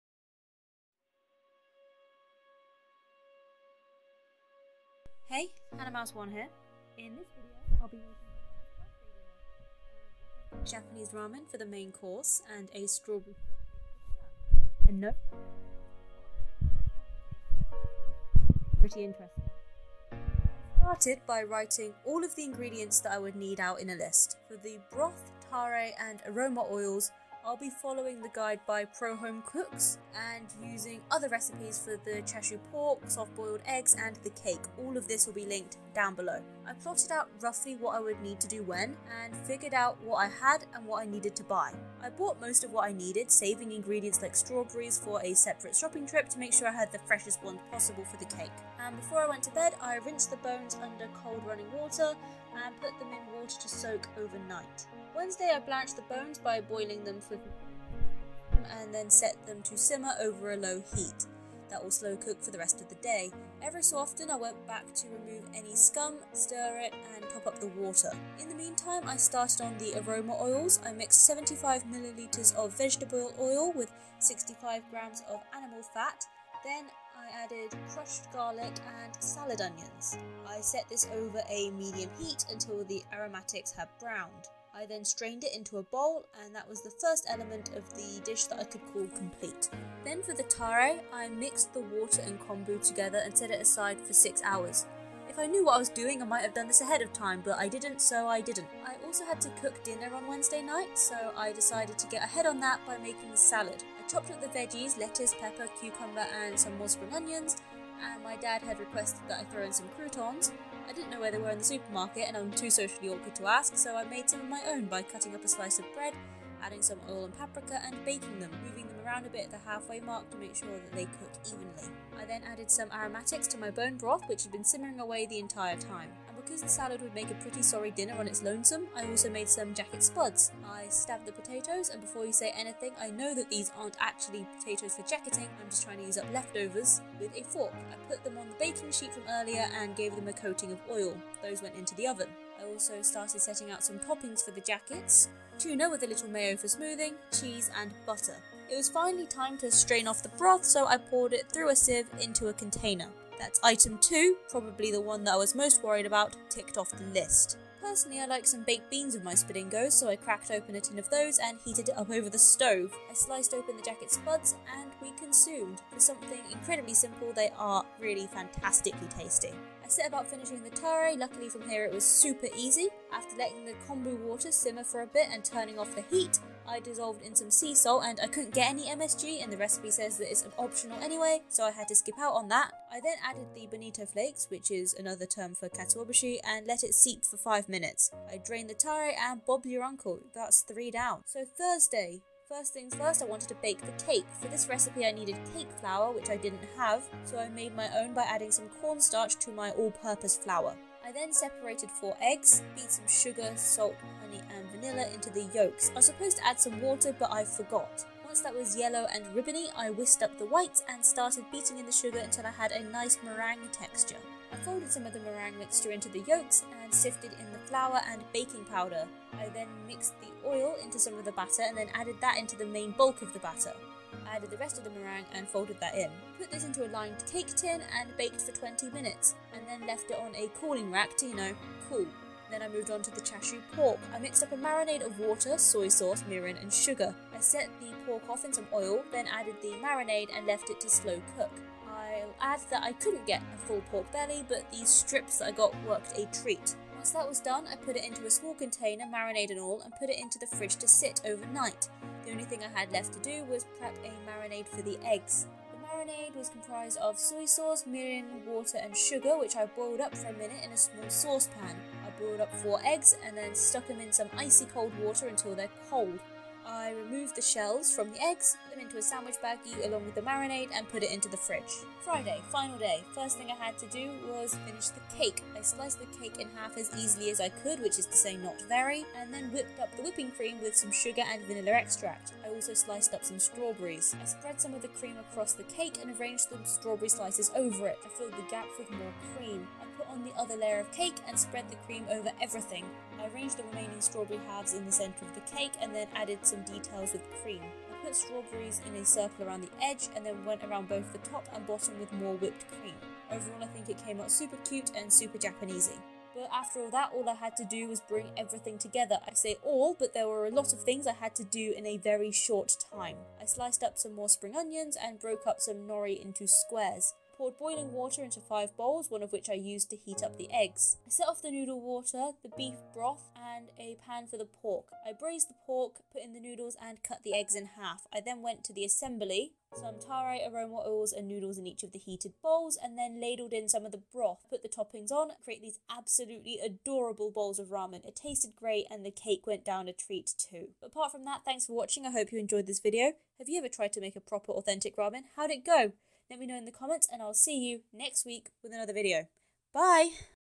Can I make my mum the PERFECT Japanese-style BIRTHDAY DINNER?
Sorry about the dodgy audio in the intro - it resolves after 30 seconds or so!

Recipes:
Broth: